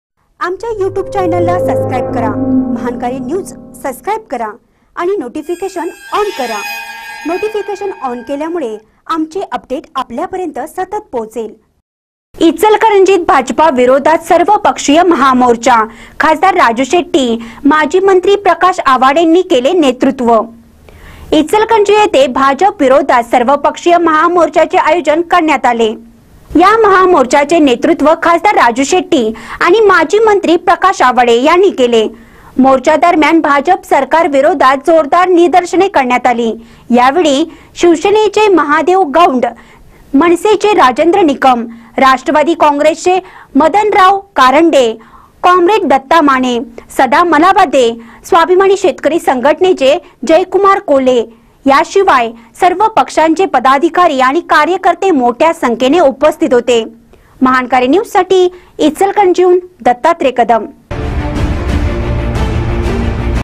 इचलकरंजीत भाजपविरोधात सर्वपक्षीय महामोर्चा
इचलकरंजीत भाजपविरोधात सर्वपक्षीय महामोर्चा : खा.राजू शेट्टी, माजी मंत्री प्रकाश आवाडेंनी केले नेतृत्व